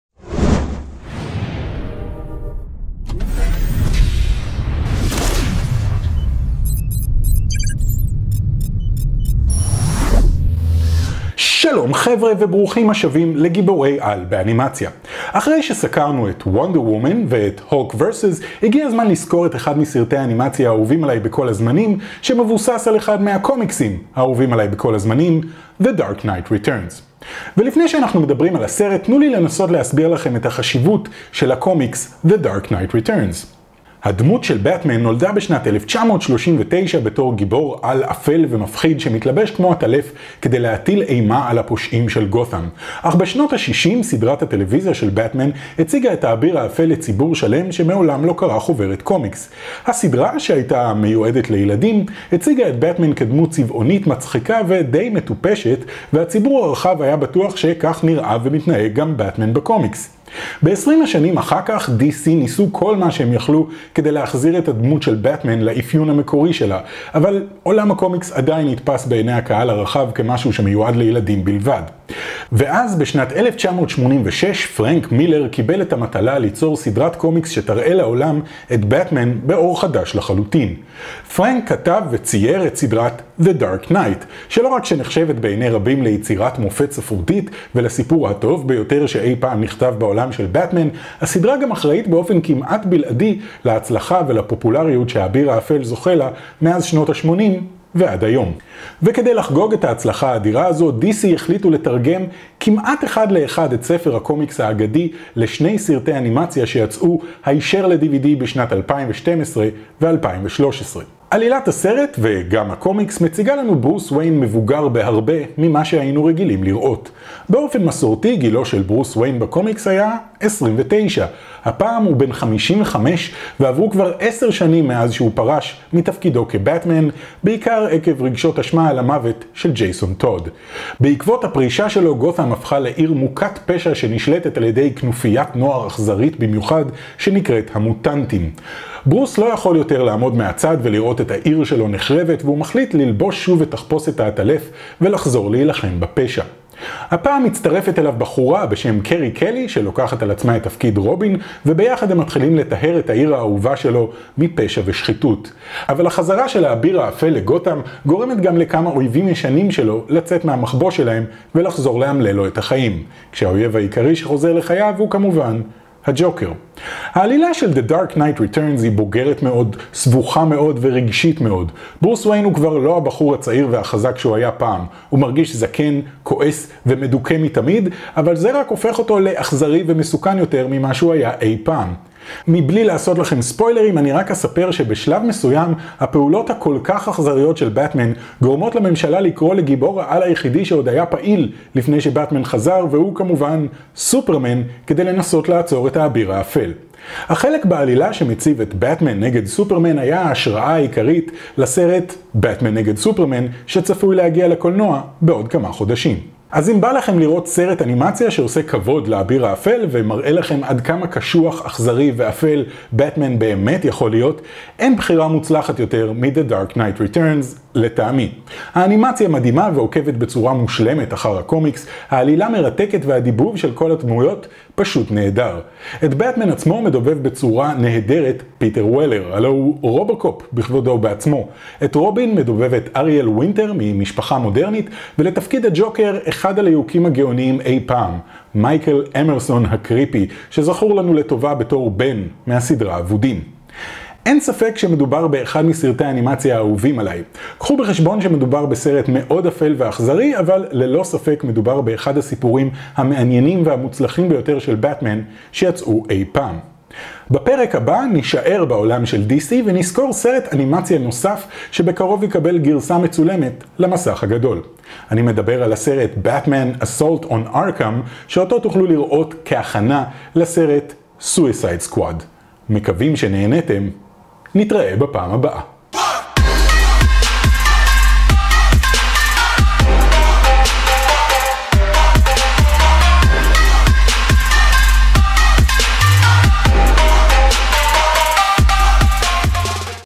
גיבורי על באנימציה - The Dark Knight Returns
פינה בה אנו סוקרים סרטי אנימציה בכיכובם של גיבורי העל האהובים עלינו.
והפעם: הסרט שמבוסס על אחד מספרי הקומיקס החשובים בכל הזמנים - The Dark Knight Returns

הכתובת לשליחת דואר לפינת גולשים שלנו:
עבור טופ גיק
ת.ד. 21642 תל אביב
מיקוד 6121601